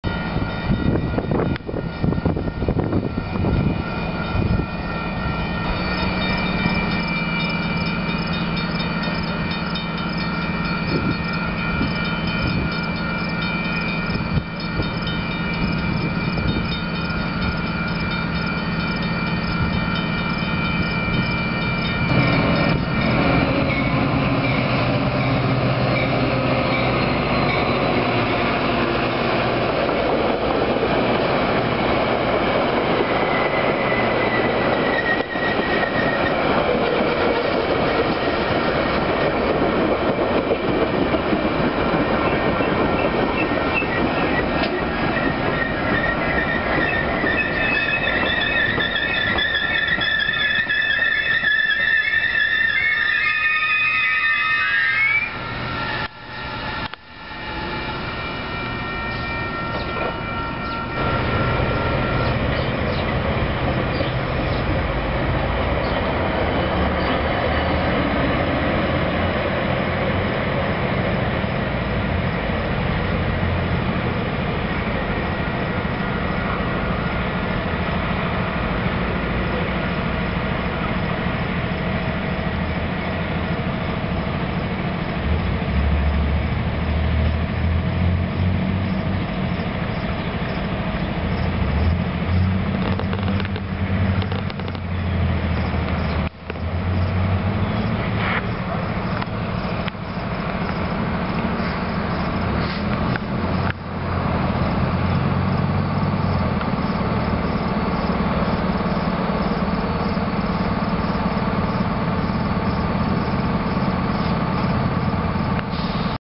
Metra UP-NW Train #703 at Woodstock, Illinois, 06.10.11
Metra Union Pacific- Northwest Line train number 703 arrives and departs Woodstock, Illinois.  This area is on UP's former Chicago & Northwestern Harvard Sub, out past the northwest suburbs.  It is served by a local freight, and at least a couple road trains run through to Chicago from Wisconsin this way.  Woodstock and the next town north, Harvard, are close enough to the greater Rockford area, and even Janesville, Wisconsin, that commuters and other Chicago-bound travellers from those areas use the stations.  Here, the train arrives Woodstock from Crystal Lake, then departs for its final stop at Harvard, about 15 minutes north.  F40PH #131, the "City of Highwood", leads 9 cars.